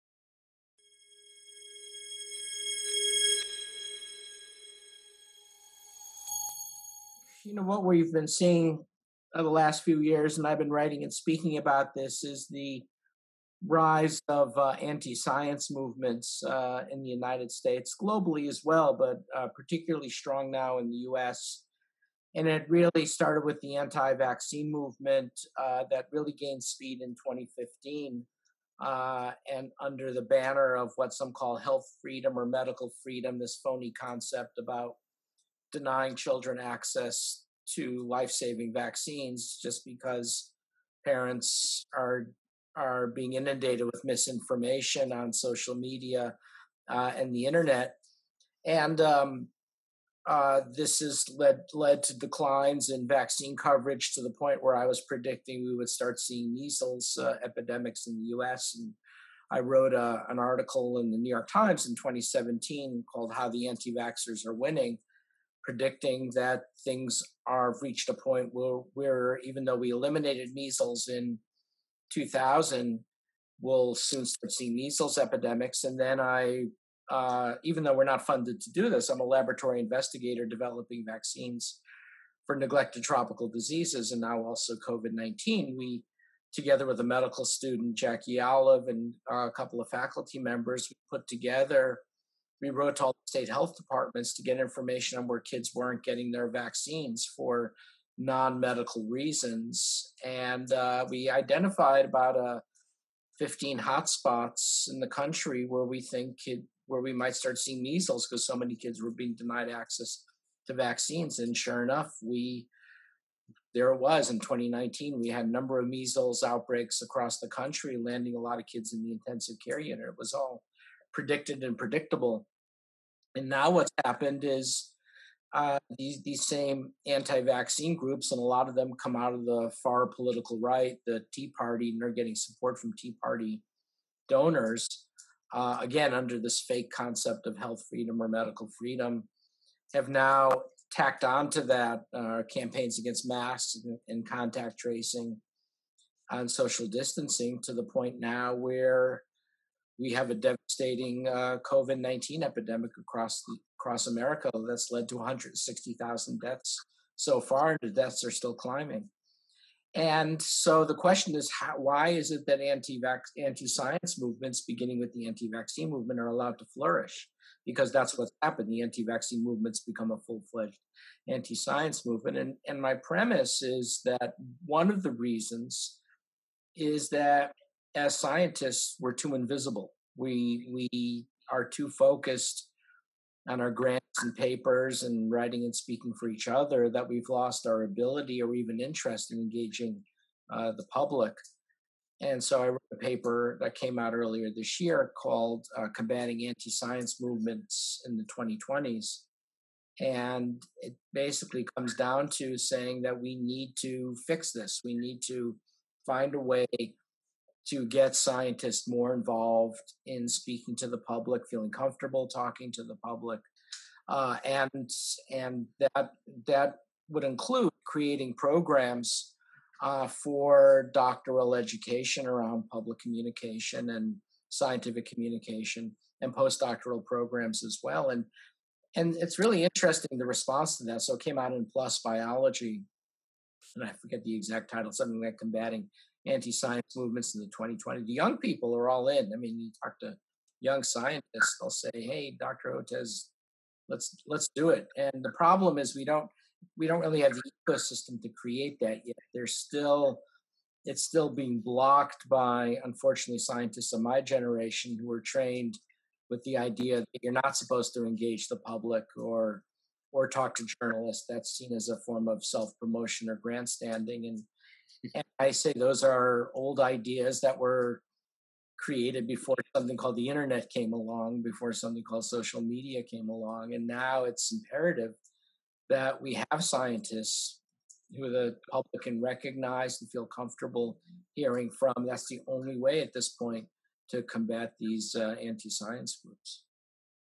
Peter Hotez: Why Experts Should Embrace Science Communication
Peter Hotez, MD, PhD, talked to Contagion about the need for scientists not to be afraid of taking a position or engaging in healthy dialogue with the public.